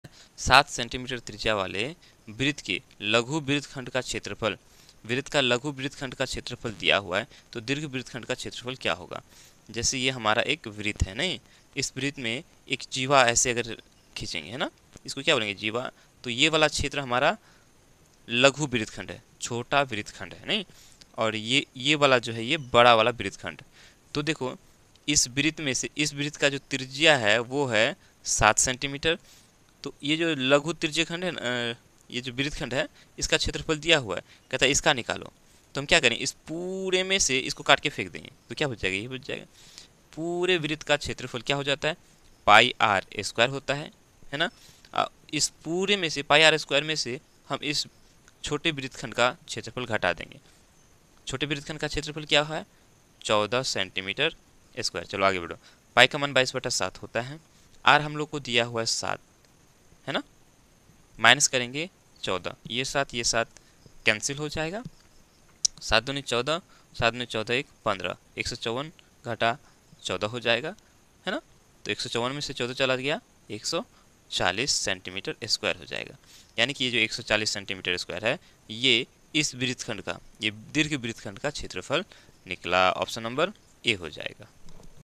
57. यदि 7 cm त्रिज्या वाले वृत्त के लघु वृत्तखंड का क्षेत्रफल 14 cm² हो, तो दीर्घ वृत्तखंड का क्षेत्
57. यदि 7 सेमी त्रिज्या वाले वृत्त के लघु वृत्तखंड का क्षेत्रफल 14 वर्गसेमी हो, तो दीर्घ वृत्तखंड का क्षेत्रफल होगा :
        (a) 140 वर्ग सेमी   (b) 150 वर्ग सेमी